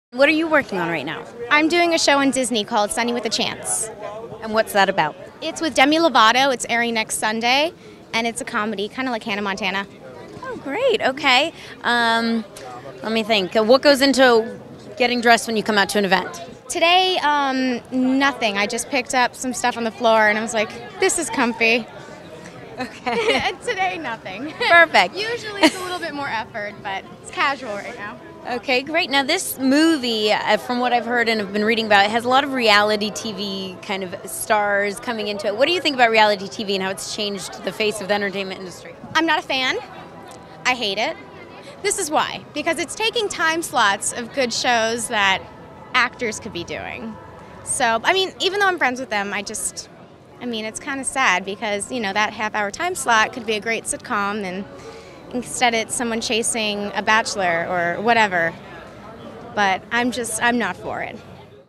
Jillian Murray on Reality TV
Exclusive interview with Jillian Murray at 2 DUDES AND A DREAM at the Arclight Theatre in Hollywood February 3, 3009 - Interview By Larissa Wohl

 © Ricomix Productions /  /